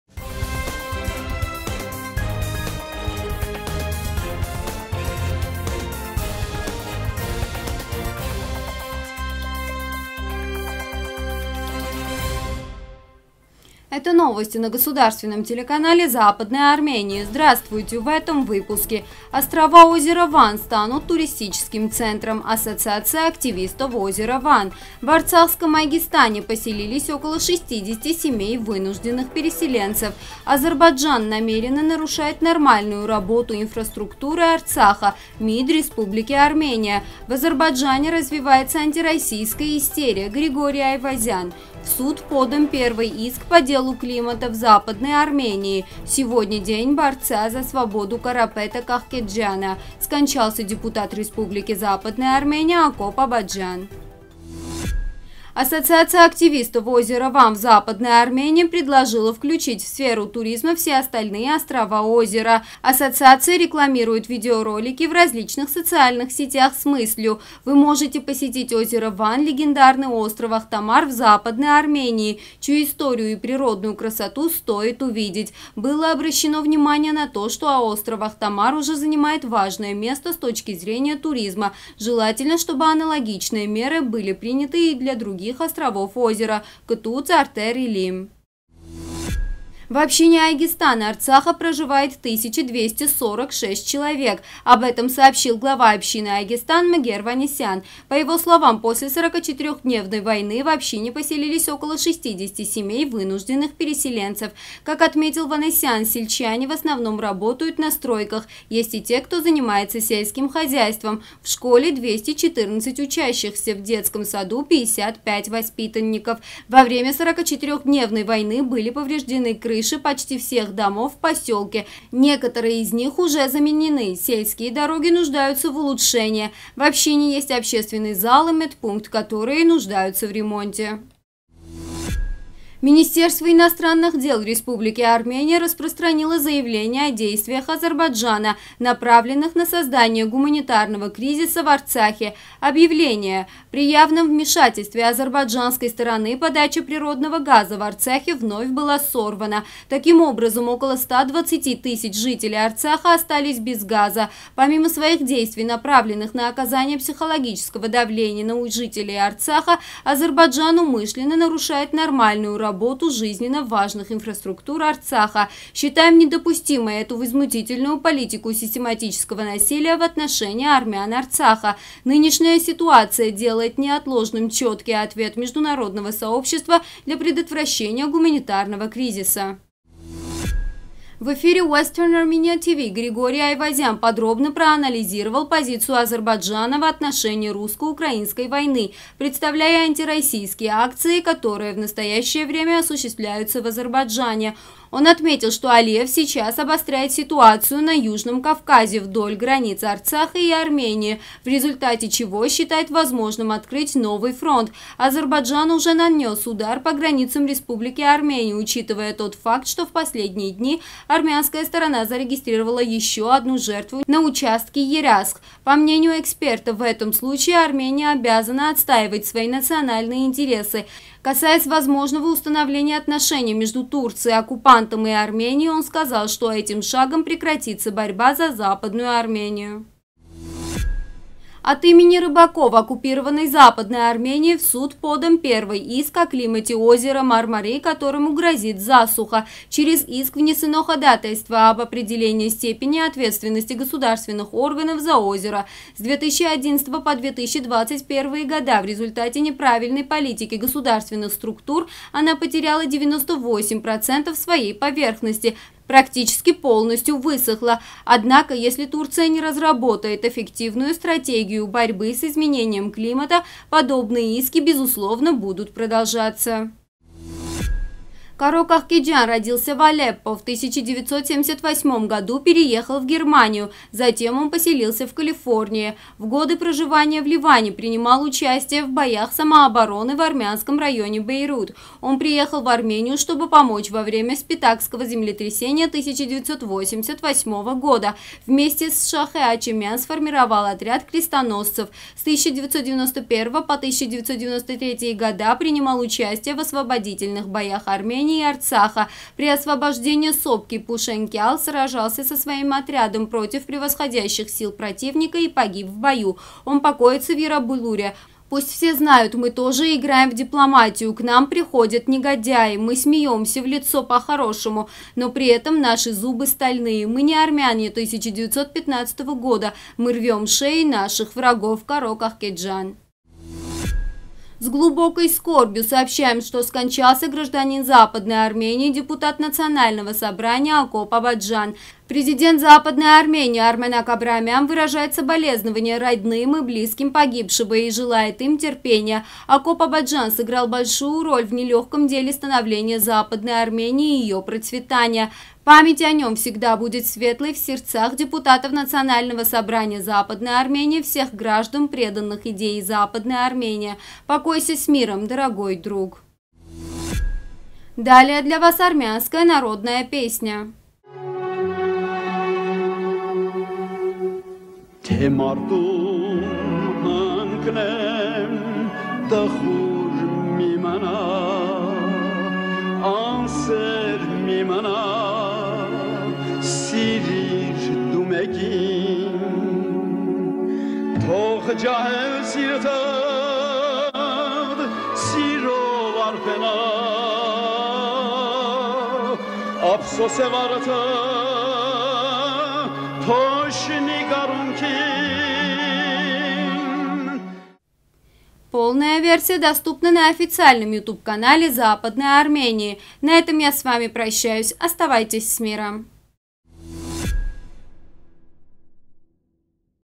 В арцахском Айгестане поселились около 60 семей вынужденных переселенцев
1․Острова озера Ван станут туристическим центром - Ассоциация активистов озера Ван.
2․В арцахском Айгестане поселились около 60 семей вынужденных переселенцев․
3․Азербайджан намеренно нарушает нормальную работу инфраструктуры Арцаха։ МИД РА․
4․В Азербайджане развивается антироссийская истерия: Григорий Айвазян.
5․В суд подан первый иск по делу о климате в оккупированной Западной Армении.
6․Сегодня день рождения борца за свободу Карапета (Каро) Кахкеджяна.
7․Скончался депутат Республики Западная Армения Акоп Абаджян.